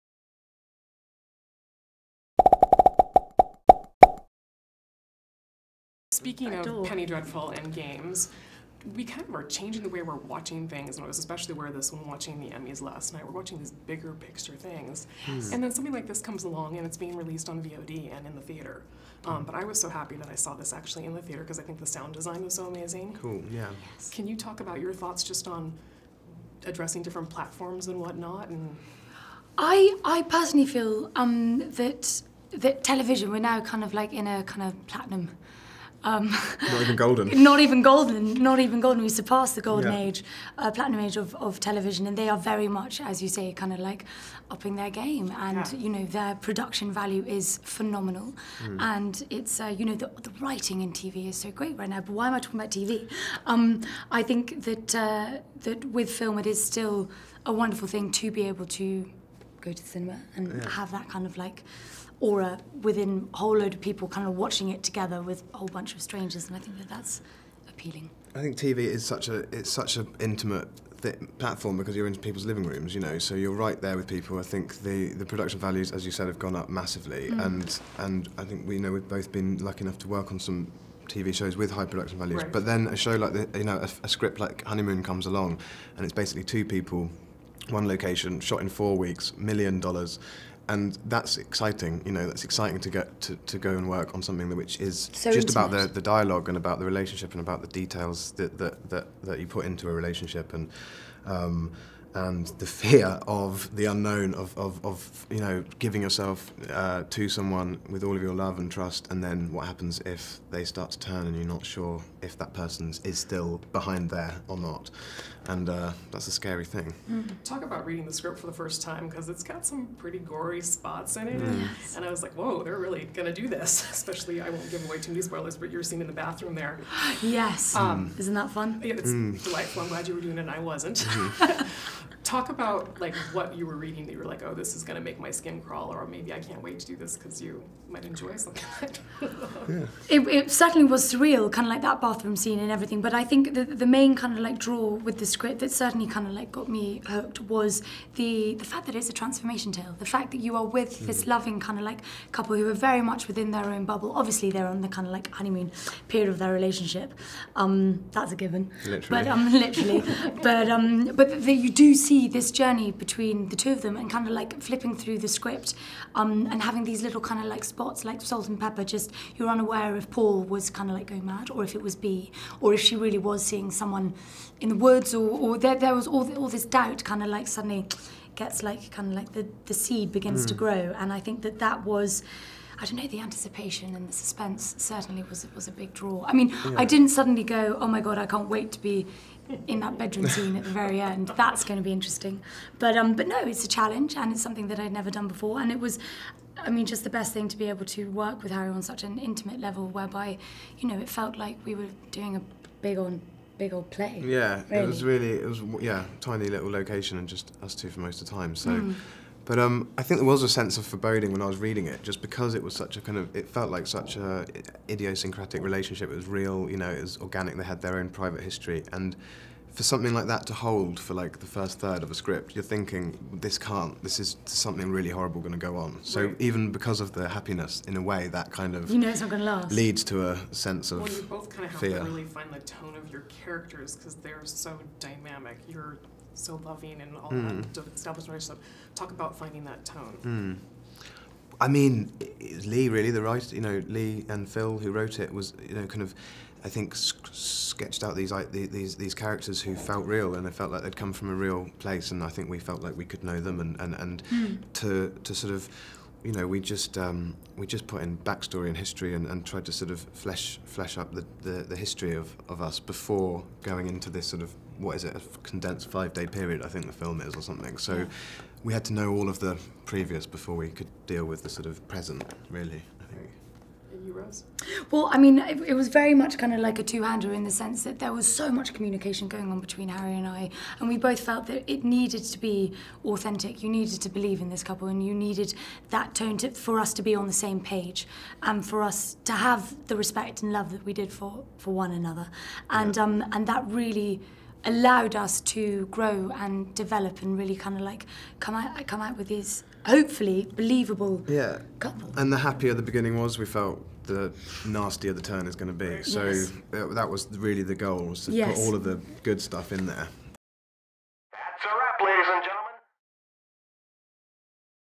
Exclusive Interview: Rose Lesie & Harry Treadaway Talk "Honeymoon"
Rose Leslie (Game of Thrones) and Harry Treadaway (Penny Dreadful) talk current entertainment platforms,  developing characters and tone of their relationship in "Honeymoon."

"Honeymoon" opens in theaters Friday, September 12th.